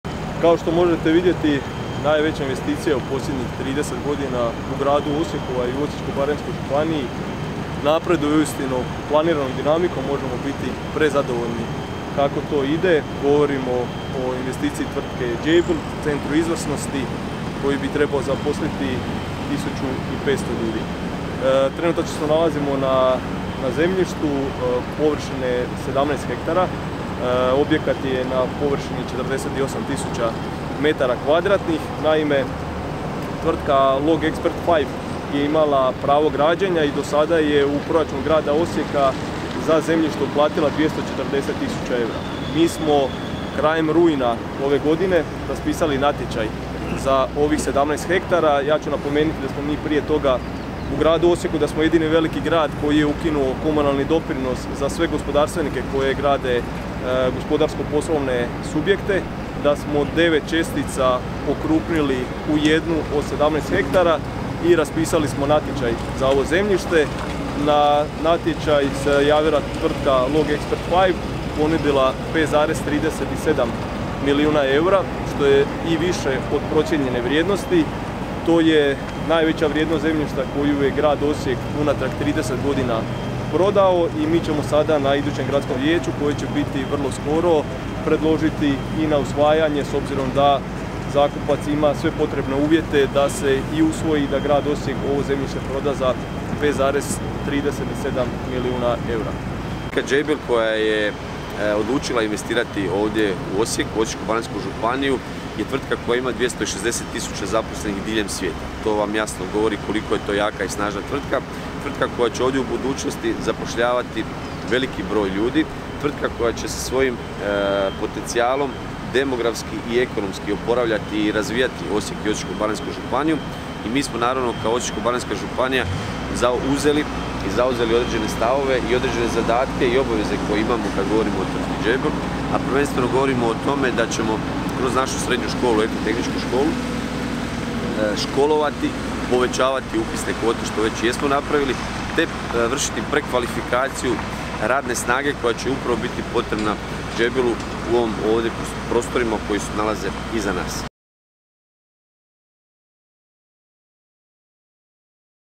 Gradilište tvrtke Jabil u Eko industrijskoj zoni Nemetin
Osijek, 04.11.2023 - Gradilište tvrtke Jabil u Eko industrijskoj zoni Nemetin. Video: Vlado Kos / CROPIX